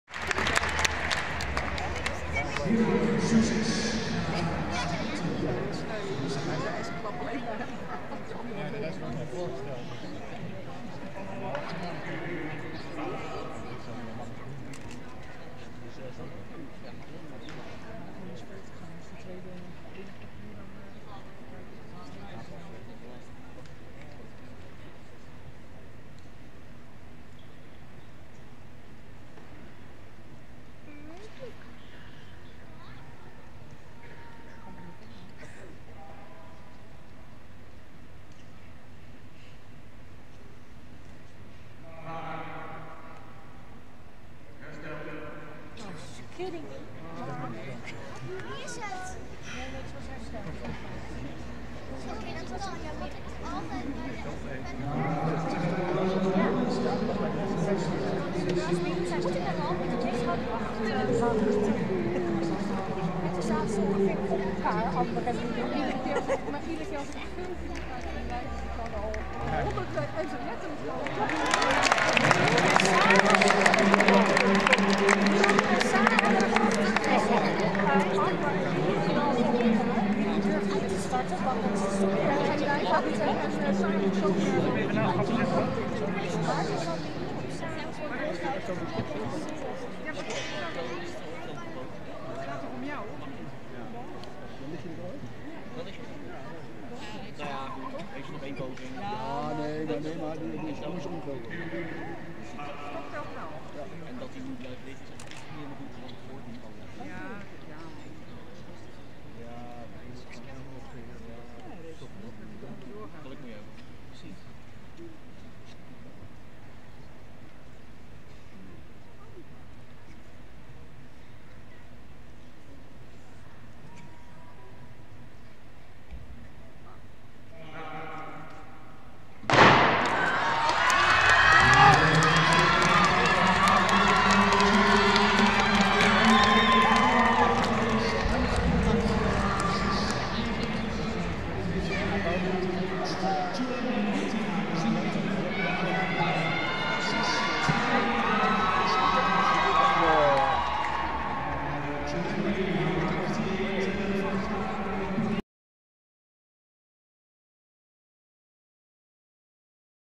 NK Indoor 2018 - 60 meter mannen serie 6 - Churandy Martina
Dutch Championships athletics. 60 meters sprint men. series 3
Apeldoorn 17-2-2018

Results:
1. Churandy Martina - 6.92s
2. Simon Blok - 7.01s
3. Rik Taam - 7.08s
4. Milo Zech Wallebroek - 7.11s
5. Sebastiaan Steenberg - 7.15s
6. Remly Vry - 7.24s
7. Kaj Emanuel - 7.24s